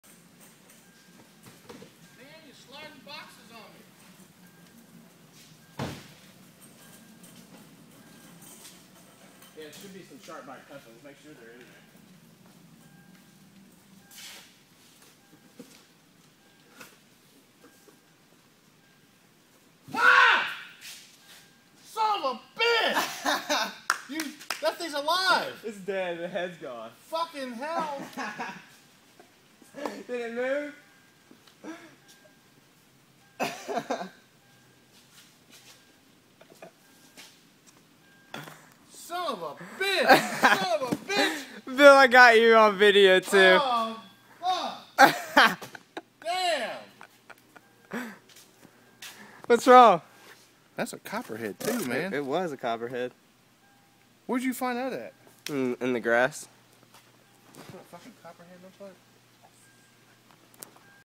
Plumbing Supply Snake Prank
Bill is shocked to find a copperhead in his box of fittings!